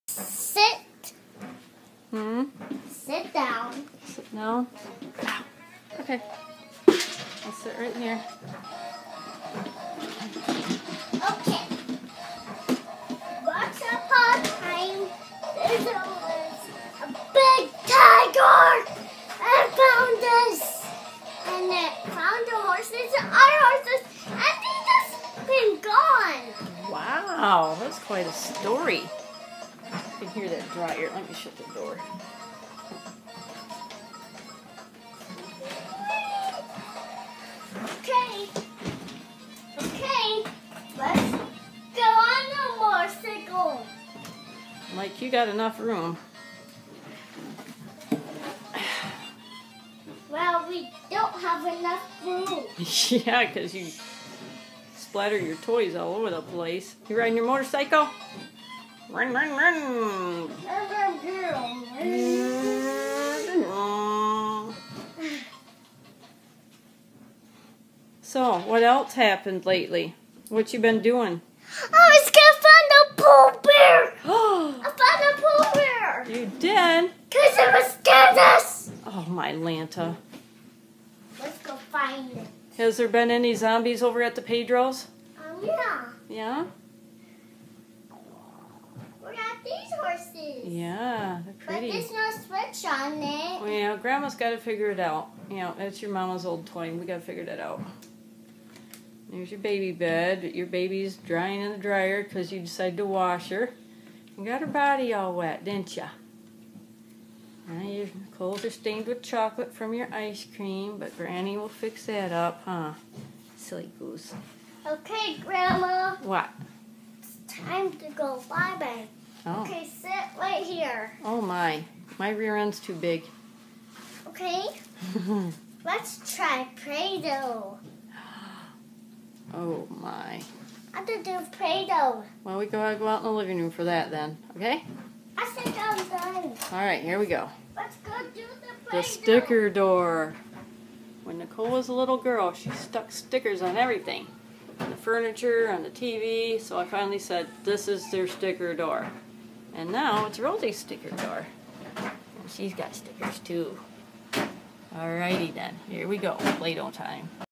Rosie Talks About A "Big Digor", Pooh Bears & Play Doh.
I asked Rosie to tell me a story, just to see what she would say. Her attention span was a bit short this time!